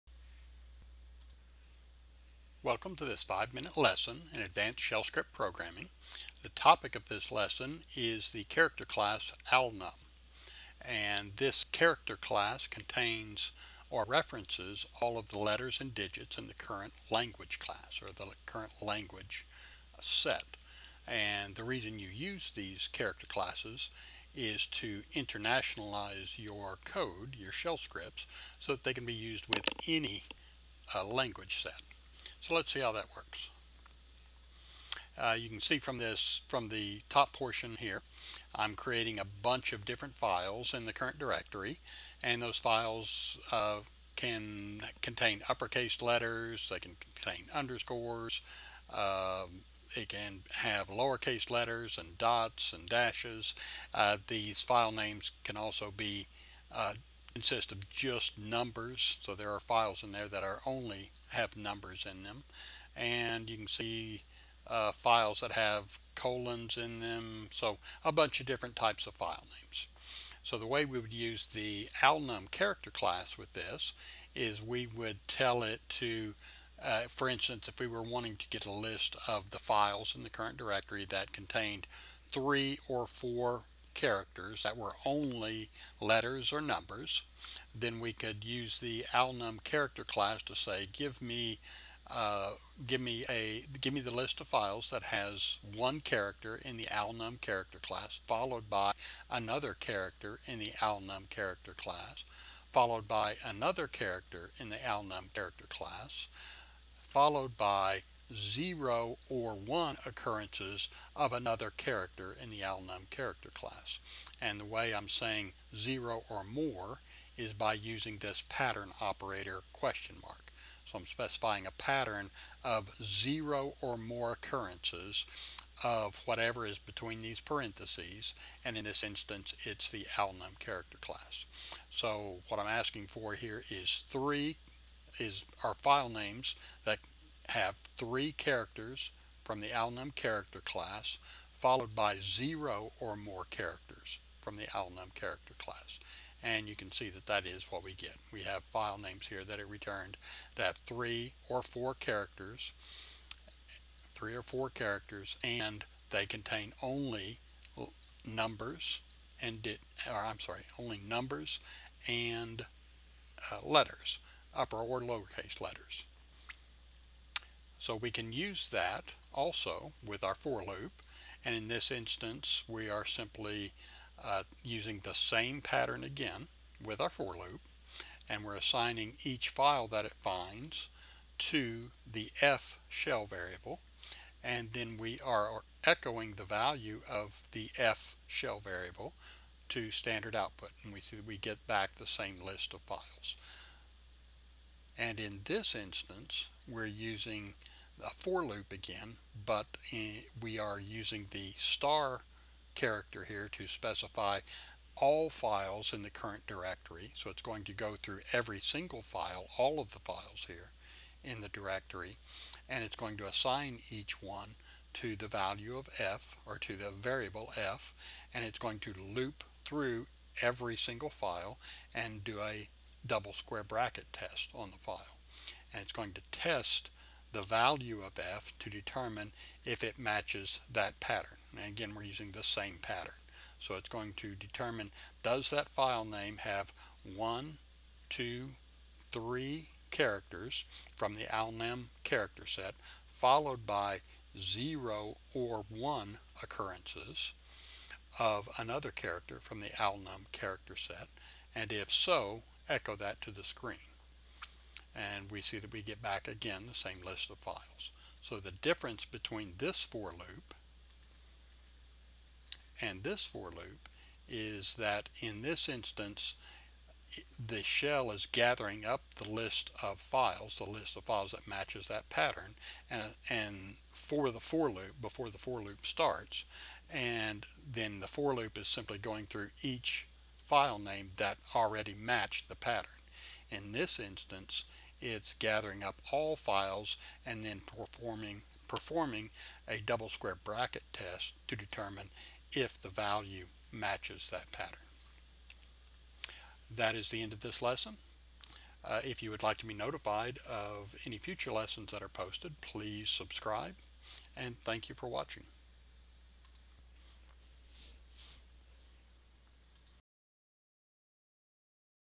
5 Minute Lessons in Shell Script Programming: Character Class [[:alnum:]]
This lesson describes the [[:alnum:]] character class and provides a few examples of how to use it.  The [[:alnum:]] character class contains all letters and digits in the current language set.